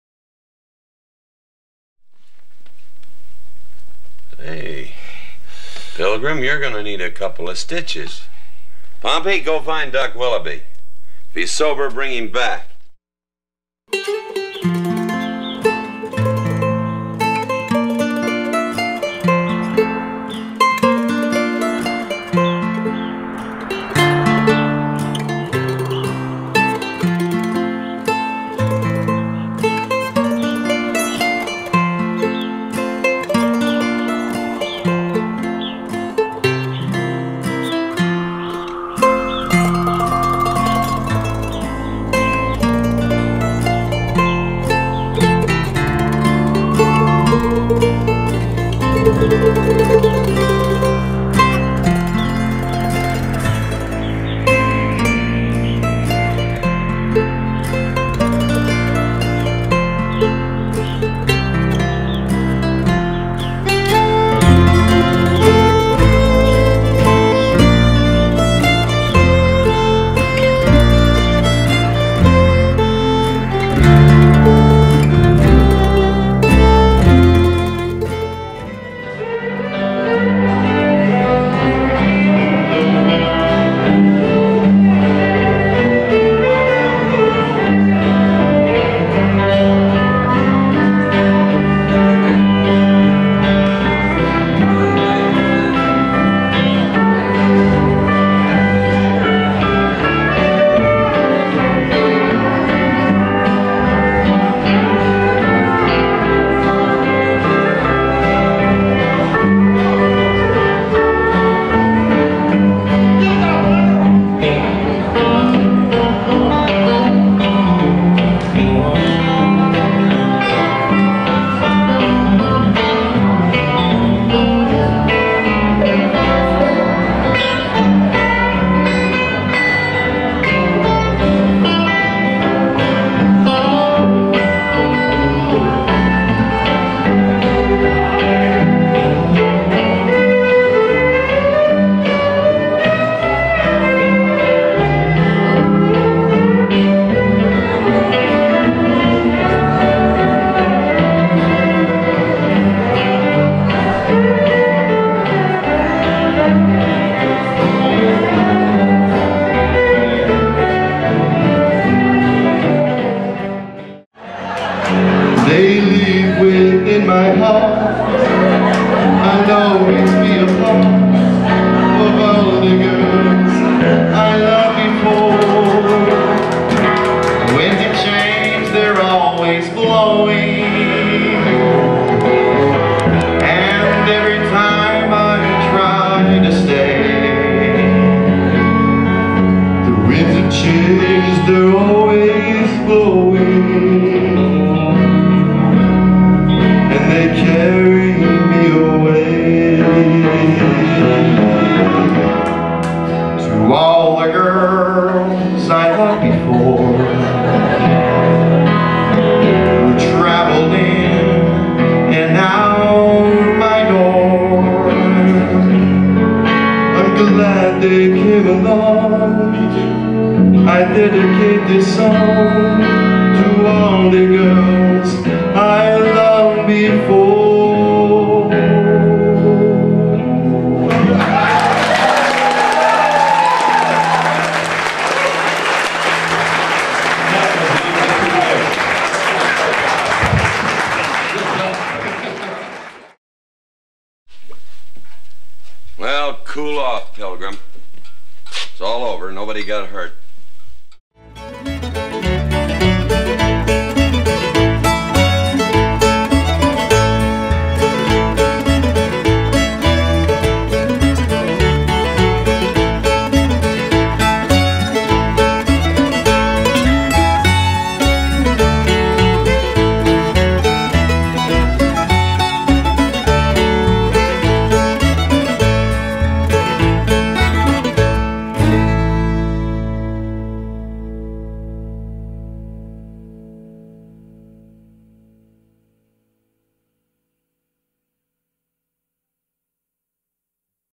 A Pilgrim,Texas Album
Images of the Pilgrim cemetery and meeting house grounds are followed by
performances by the Pilgrim Heirs Band and by special guest, John Wayne Schulz.